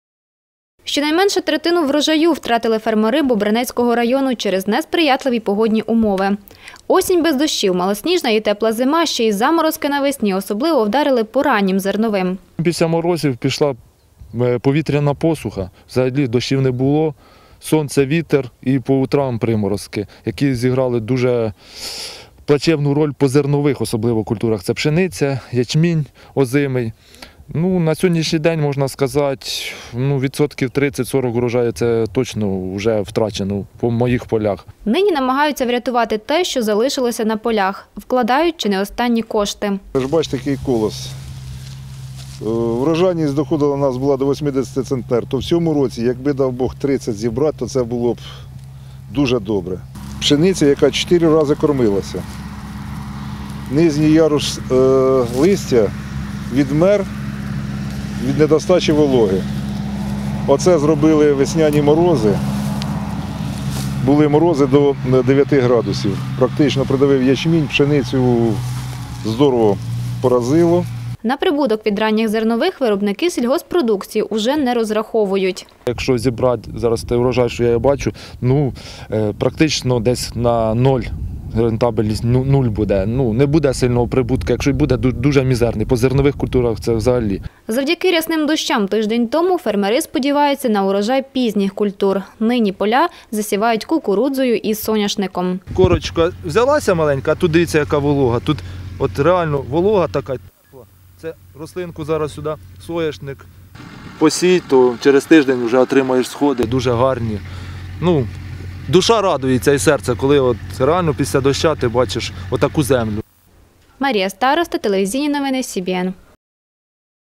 Третину врожаю втратили фермери Бобринецького району | телеканал Вітер
Щонайменше третину врожаю втратили фермери Бобринецького району через несприятливі погодні умови. Осінь без дощів, малосніжна і тепла зима ще й заморозки навесні особливо вдарили по раннім зерновим.